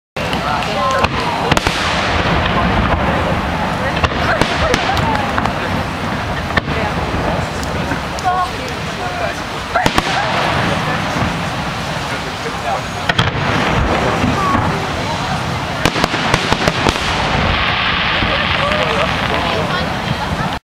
Fireworks at the Alster
Little glimpse of this year's fireworks at the Japenese Kirschblütenfest in Hamburg.